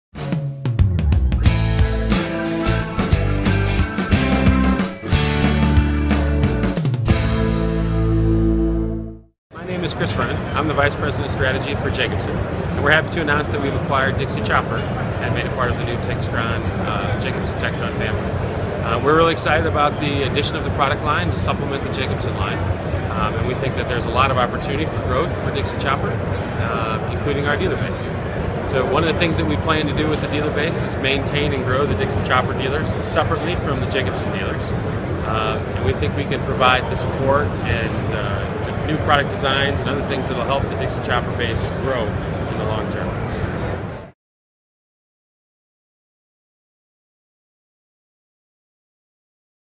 Message to Dealers From Jacobsen Textron on Acquisition of Dixie Chopper
We met with Chris Vernon of Jacobsen Textron to talk about the recent announcement of the company's acquisition of Dixie Chopper.